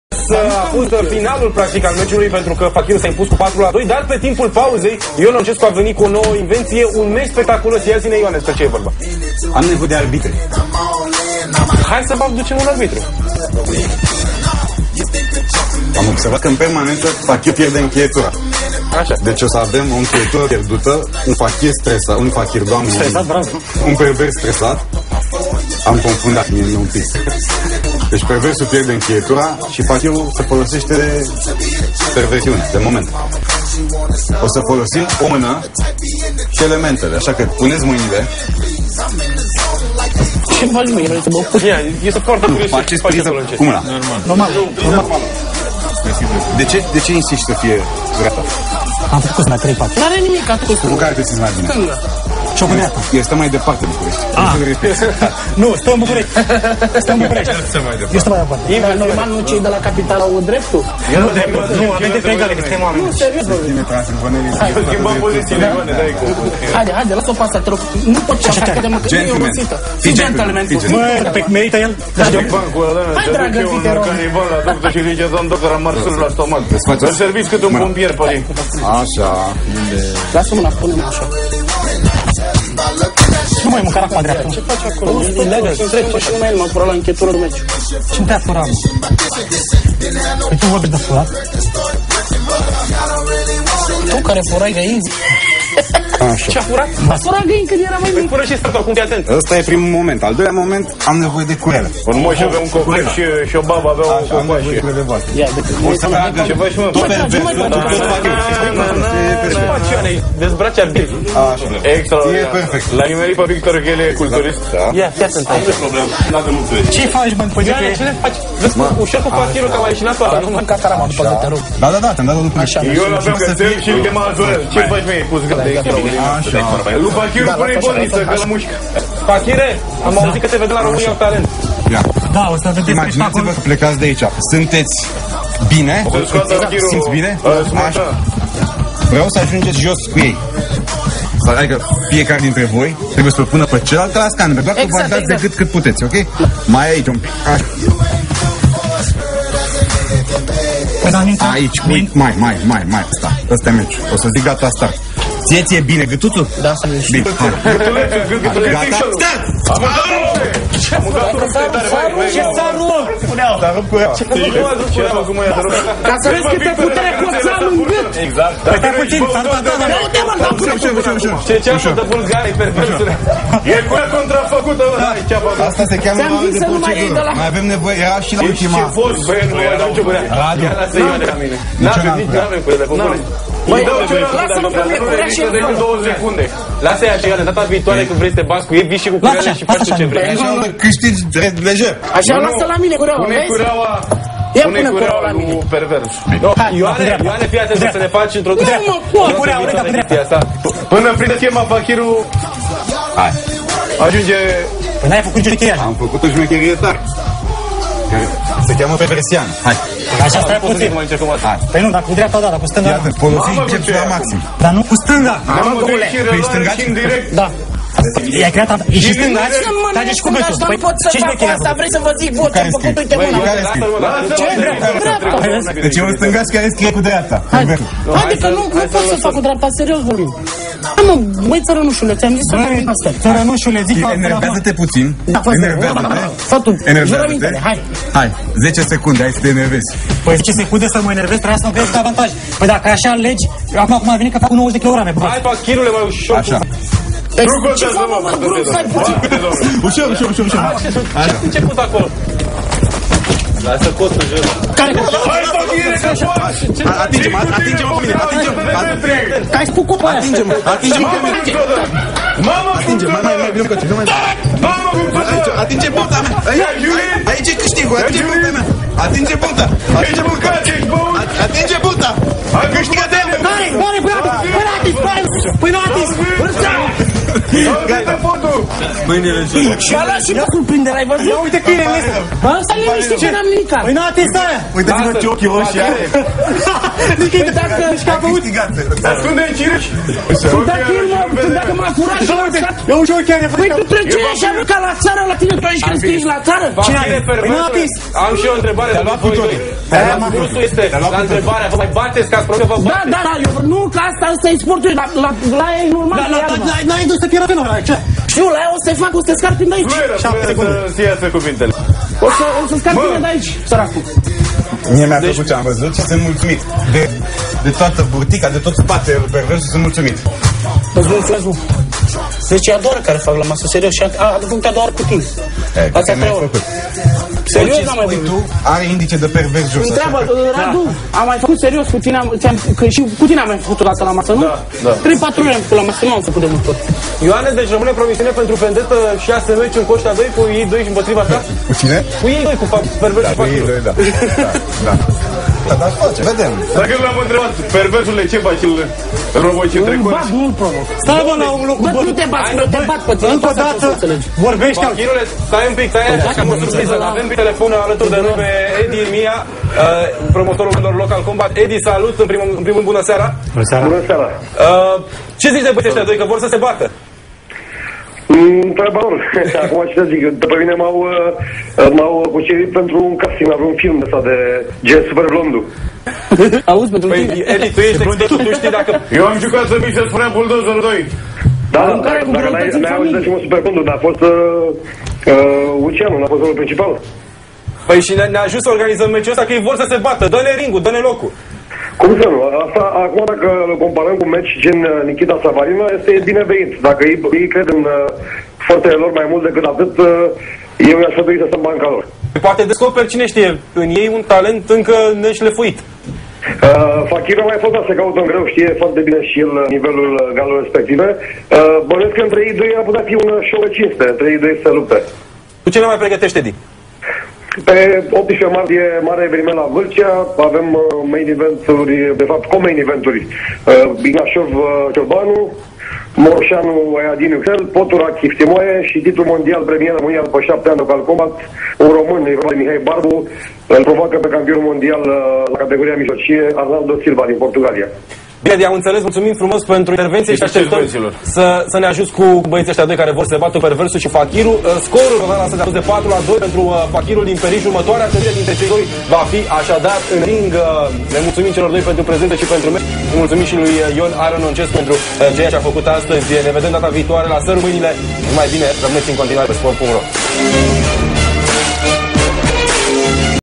Perversu vs Fachiru Revansa 28.02.2011 Partea 5 (live de pe sport.ro)
- sponsor in cadrul meciului de skandenberg dintre Fachiru si Perversu Revansa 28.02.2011 Partea 4 (live de pe sport.ro)